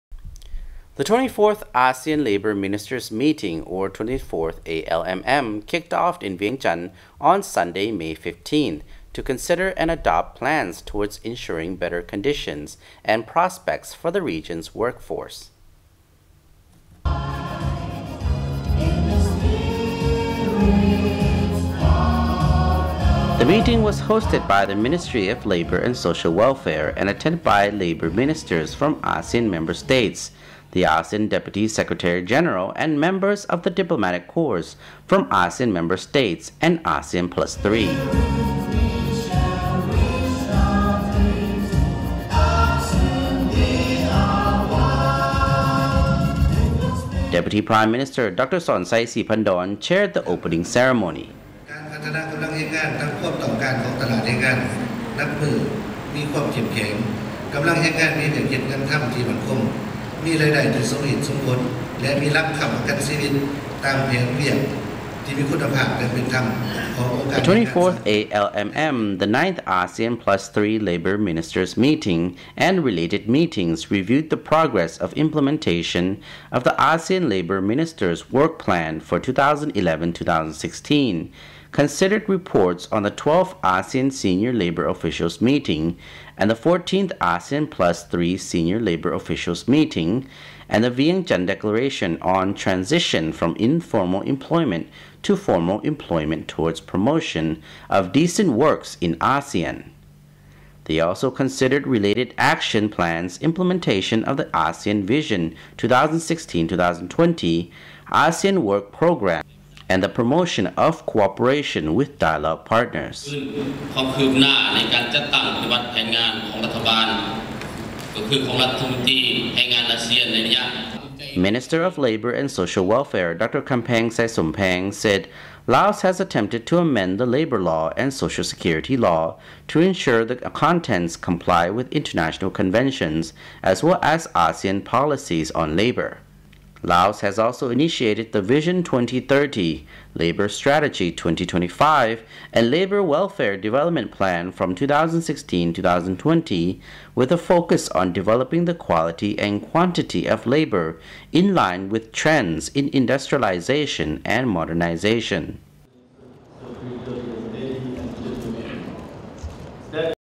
Lao NEWS on LNTV: The 24th ALMM kicks off in Vientiane on Saturday May 15th.16/5/2016
VO The 24th Asean Labour Ministers Meeting (24th ALMM) kicks off in Vientiane on Saturday May 15th
INTRO: The 24th Asean Labour Ministers Meeting (24th ALMM) kicked off in Vientiane on Sunday May 15th  to consider and adopt plans towards ensuring better conditions and prospects for the region's workforce.
STORY: The meeting was hosted by the Ministry of Labour and Social Welfare and attended by labour ministers from Asean member states, the Asean Deputy Secretary-General and members of the diplomatic corps from Asean member states and Asean Plus 3.
Deputy Prime Minister Dr Sonexay Siphandone chaired the opening ceremony.
The 24th ALMM, the 9th Asean Plus Three Labour Ministers Meeting and related meetings reviewed the progress of implementation of the Asean Labour Ministers Work Plan from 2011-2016, considered reports on the 12th Asean Senior Labour Officials Meeting and the 14th Asean Plus Three Senior Labour Officials Meeting, and the Vientiane Declaration on “Transition from Informal Employment to Formal Employment towards Promotion of Decent Works in Asean”. They also considered related action plans, implementation of the Asean Vision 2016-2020, Asean work programmes, and the promotion of cooperation with dialogue partners.
Minister of Labour and Social Welfare Dr Khamphaeng Xaysomphaeng said Laos has attempted to amend the Labour Law and Social Security Law to ensure the contents comply with international conventions as well as Asean policies on labour.
Laos has also initiated the Vision 2030, Labour Strategy 2025 and labour and welfare development plan from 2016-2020 with a focus on developing the quality and quantity of labour in line with trends in industrialisation and modernisation.